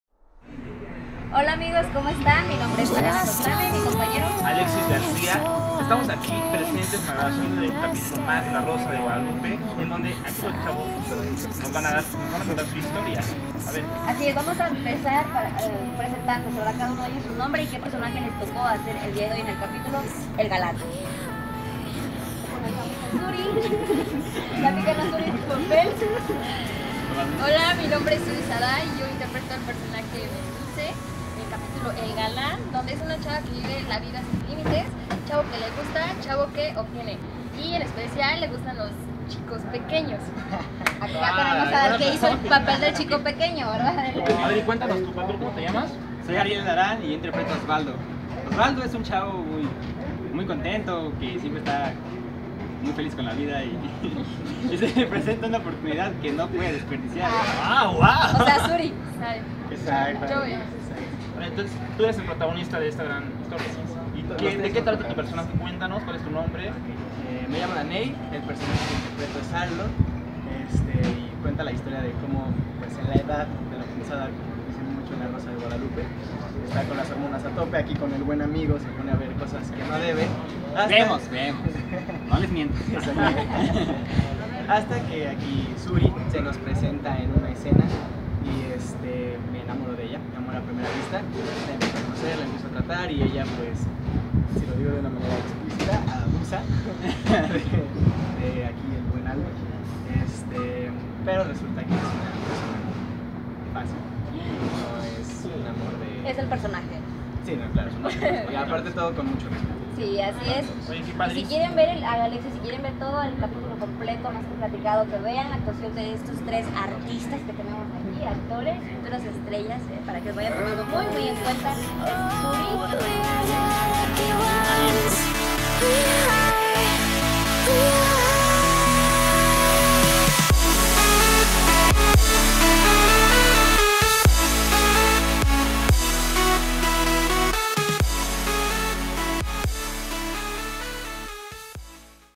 Entrevista a actores de La Rosa de Guadalupe
Conoce a jóvenes de La Rosa de Guadalupe, actores del capítulo "El galán".
Entrevistadores: Malena Coclame y Alexis García.